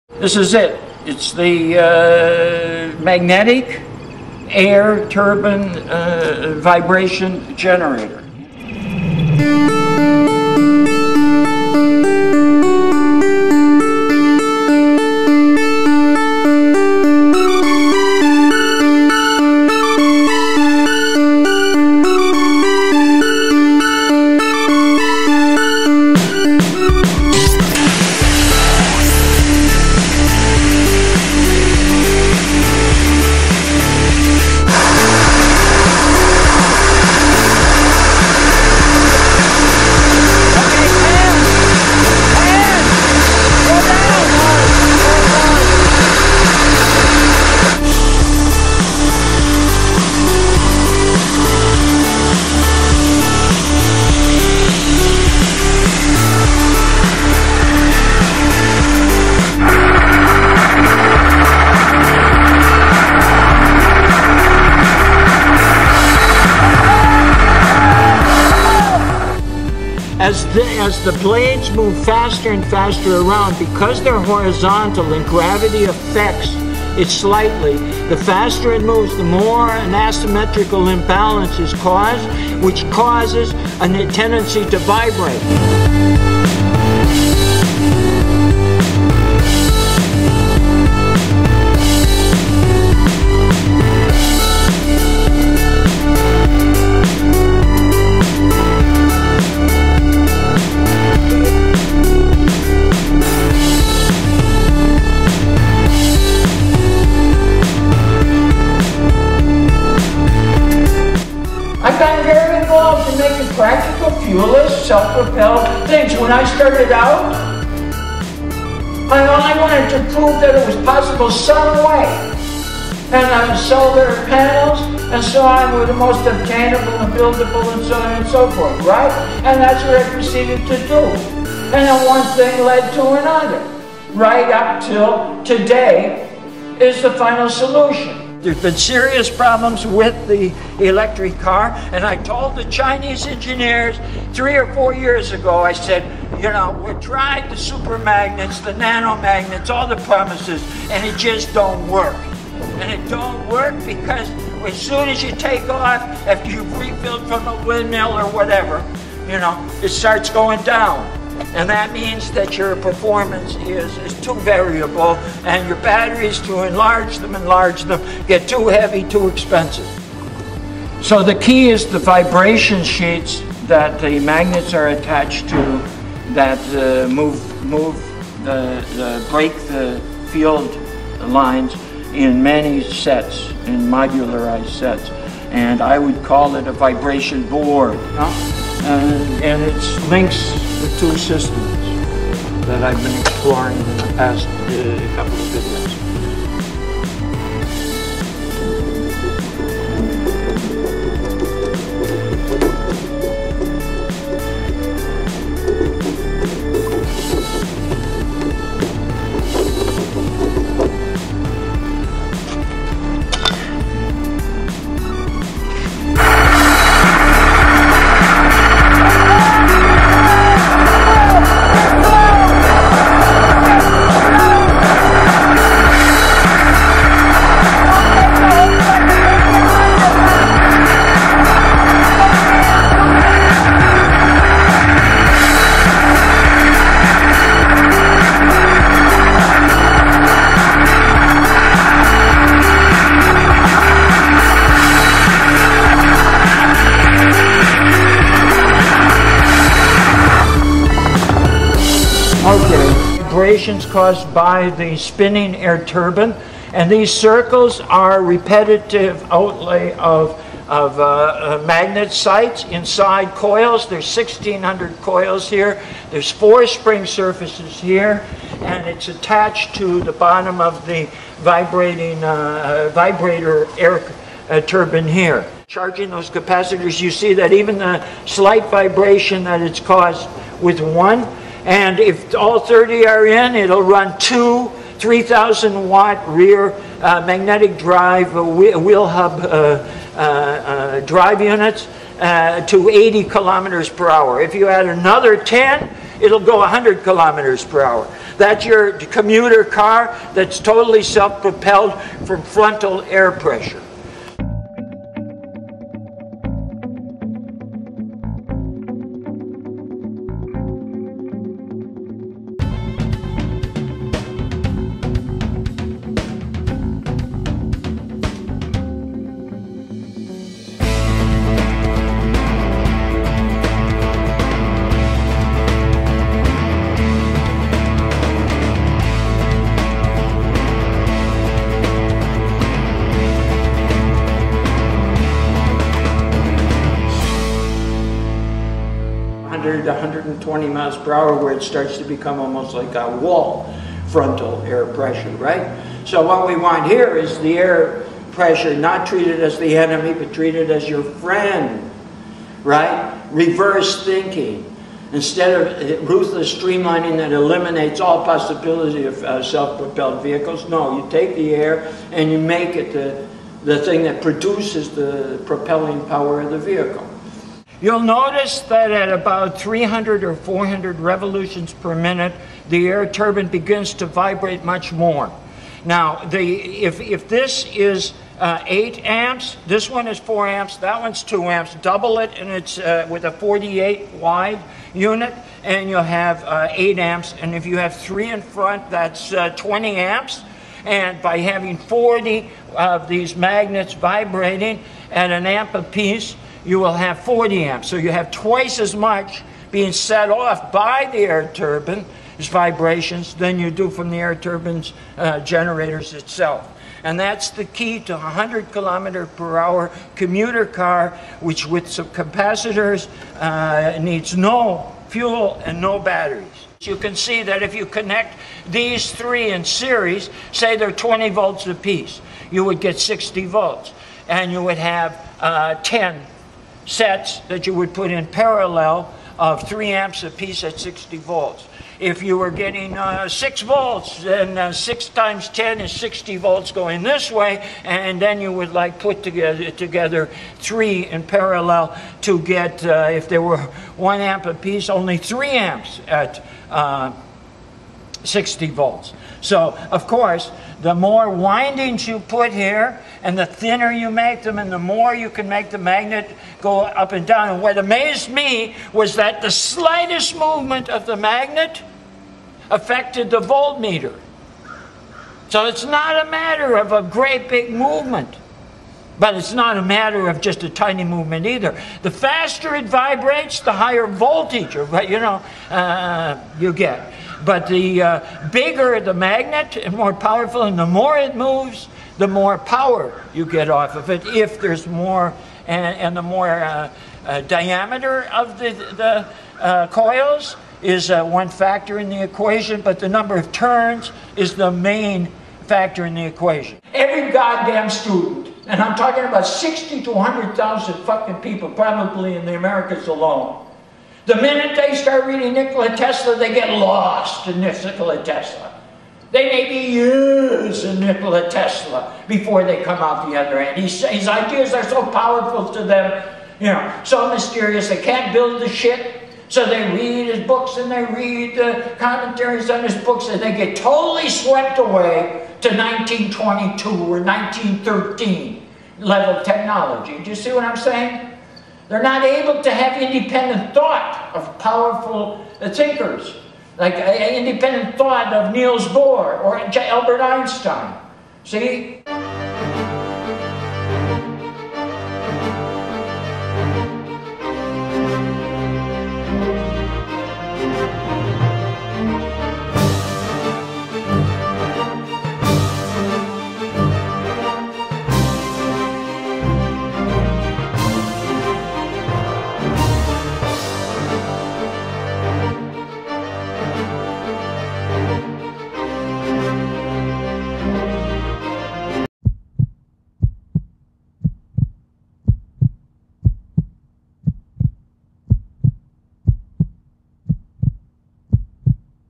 MAG AIR VIBE GEN
Magnetic  Air Turbine Vibration Generator 1

The first Magnetic Air Turbine and Vibration board as dual Generator

Provides increased generating power for Self-Propelled Transport

First test of 4-amp Air Turbine reached 40v as linked Vibration board with single unit reached 6.19 V.

Vehicular frontal air pressure generates energy directly and generates vibrational energy  which further powers the vehicle

This system is scalable: Mag Air Gen - 1 was 24"W and put out 2amps at 36v at 400r/p/m's.

This vids 34" Turbine produced 4amps at 40v at 420r/p/m's; so it is logical that 48" Turbine will produce 8amps at 48v at 460r/p/m's. Two of these placed where before were "Bumper" & "radiator" of old fuel-motor "cars", plus one like this one tested along roof-line, will produce 20amps - and perhaps 40amps from improved resonators. This is enough to drive 2 3000w Magnetic Drives on the rear wheels to 100km/p/hr, and up an incline of about 22*.

Note: These two tests when compared roughly indicated a one volt increase for each millimeter of magnet vibration (given here 1600 turns of no.32 wire & 1 1/2" 15000guass magnet). This makes commuter vehicle power demands fairly simple, i.e. 60 such combinations made to move 60 millimeters ea. will drive such a vehicle to 100km/p/hr. This does not include supplements such as Air Turbines, Solar accumulators, or others.

Several resonator designs are being worked on in various countries: Ribbon types / coils vibrating between two magnetic opposite polarity sheets and others. See for example "theEnergyDream" channel on YouTube. Rapid progress is to be expected, all within surprisingly low cost & material parameters.

GOOGLE:
"Mag Amp Gen"
"Mag Air Gen"
"Mag Vibe Gen"